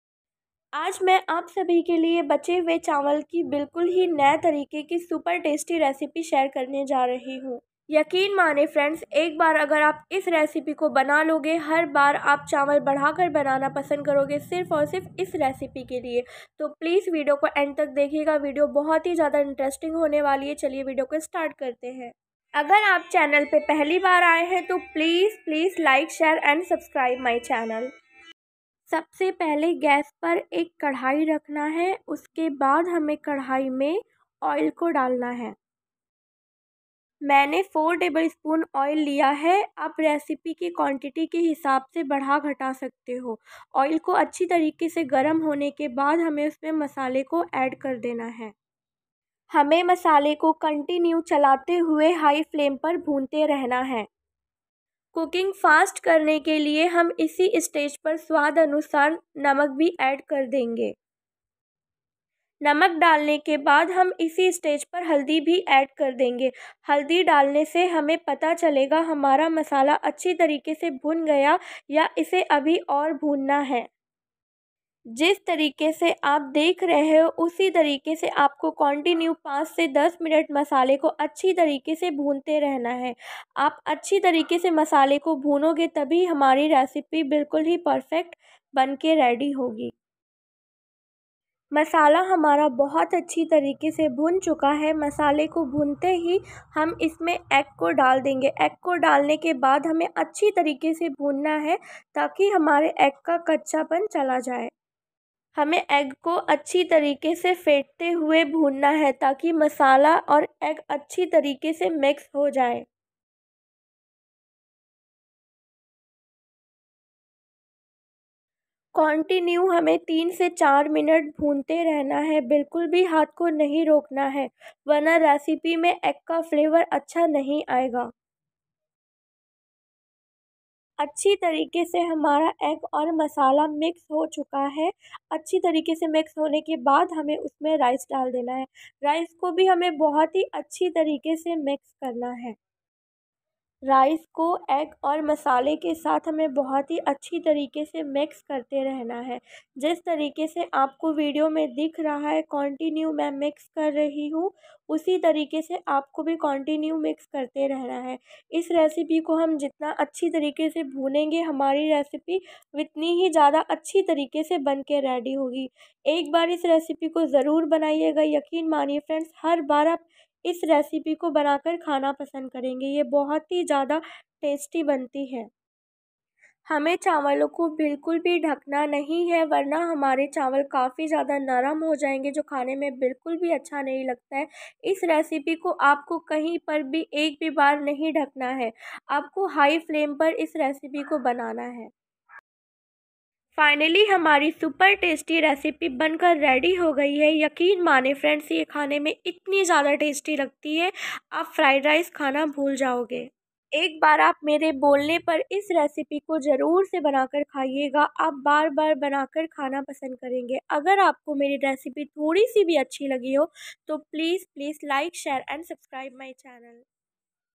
बचे हुए चावल की बिल्कुल ही नए तारिके की रेसिपी गारंटी है ना आपने कभी खाया होगा ना कहीं देखा होगा
hi I am Khushi aaj main aap sabhi ke liye bilkul hi naye tarike ki recipe lekar I hun bache hue chawal ki naye tarike ki recipe ek bar is recipe ko jarur TRAI Karen I am sure aapko recipe bahut hi jyada pasand aaegi agar aapko meri recipe thodi si bhi pasand aaye To please please like share and subscribe my channel Khushi kitchen experiment bache hue chawal ki recipe